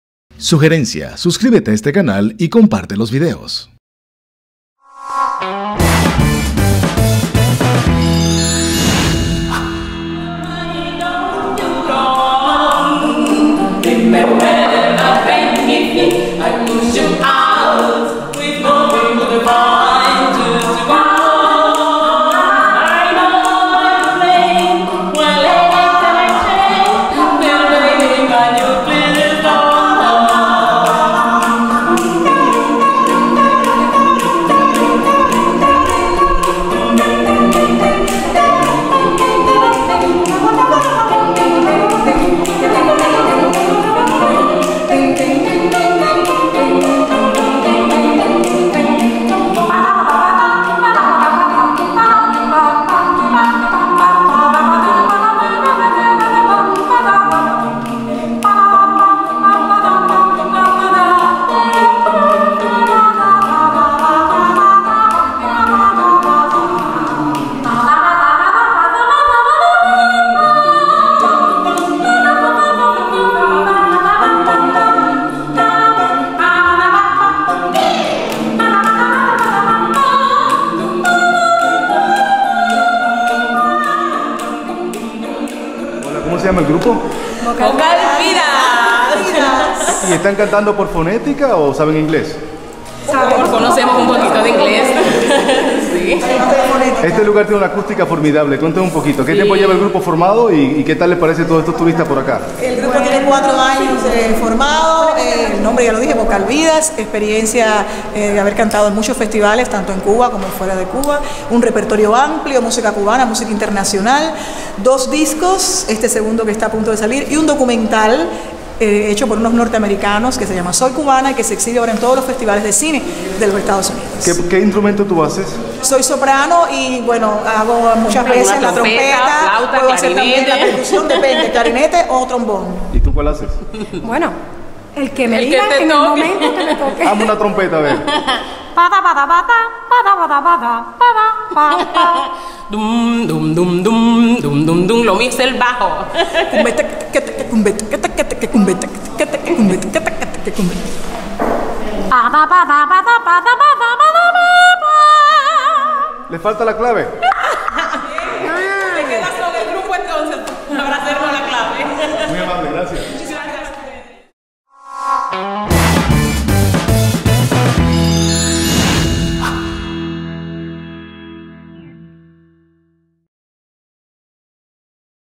Quinteto Cubano "Vocal Divas"
Esta agrupación vocal femenina surge en Abril del año 2000 en la ciudad de Santiago de Cuba por iniciativa de seis jóvenes cantantes, integrantes de los coros profesionales de la ciudad.
Actualmente la integran cuatro muchachas con graduaciones en Dirección Coral, Educación Musical, Canto y Filología, quienes poseen además una vasta experiencia en corales de escuelas de música, universitarias y profesionales.